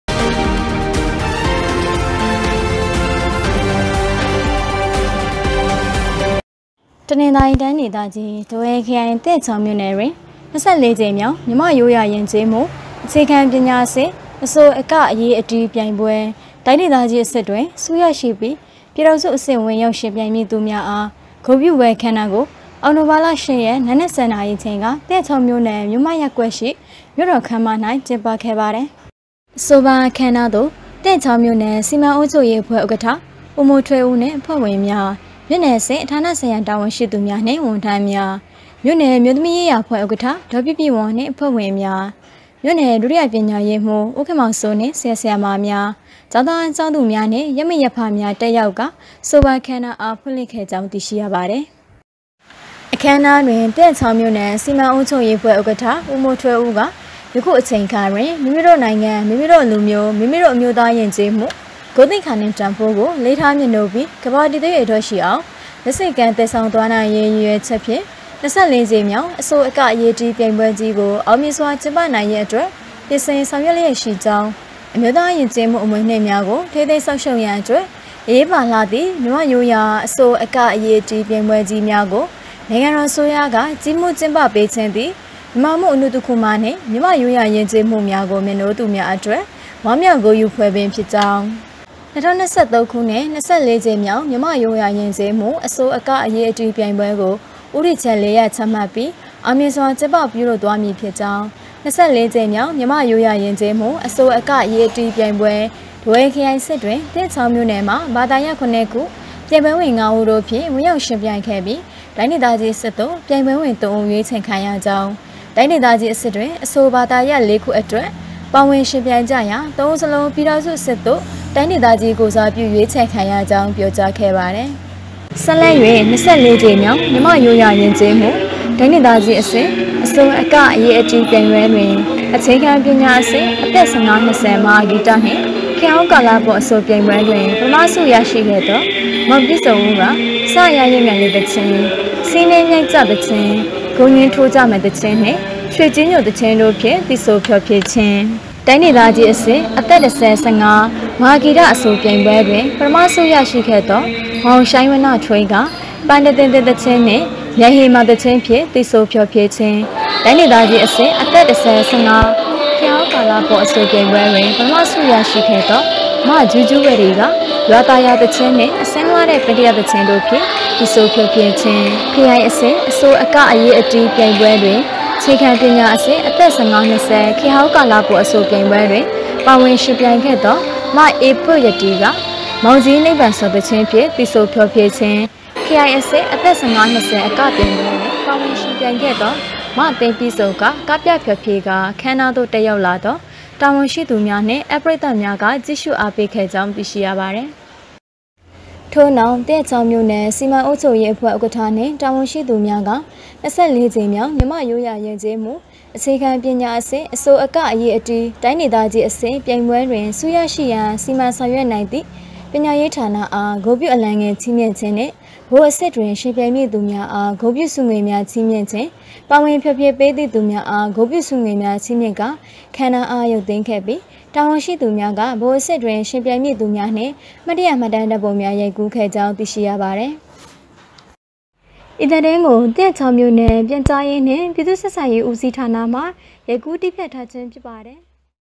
မြန်မာ့ရိုးရာယဉ်ကျေးမှု အဆို၊အက၊အရေး၊အတီးပြိုင်ပွဲ ပြည်ထောင်စုအဆင့်ပြိုင်ပွဲဝင်မည့်သူများအားဂုဏ်ပြု
သရက်ချောင်းမြို့နယ်၌ (၂၄)ကြိမ်မြောက်၊ မြန်မာ့ရိုးရာယဉ်ကျေးမှု အဆို၊ အက၊ အရေး၊ အတီး ပြိုင်ပွဲ ပြည်ထောင်စုအဆင့် ပြိုင်ပွဲဝင်မည့်သူများအား ဂုဏ်ပြုပွဲအခမ်းအနားကျင်းပ
သရက်ချောင်း အောက်တိုဘာ ၁၂
မြို့နယ်